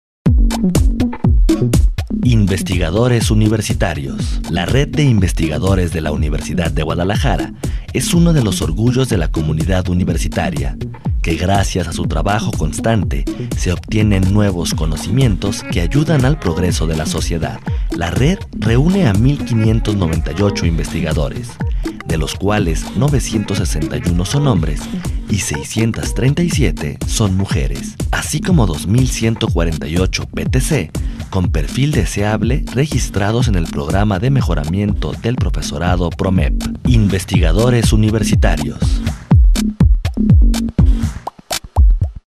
CAP INVESTIGADORES Universidad de Guadalajara 01 - Marzo 21 de 2013
COBERTURA ESPECIAL del Informe de Actividades del Rector General de la Universidad de Guadalajara, Doctor Marco Antonio Cortés Guardado.
Jueves 21 de Marzo del 2013.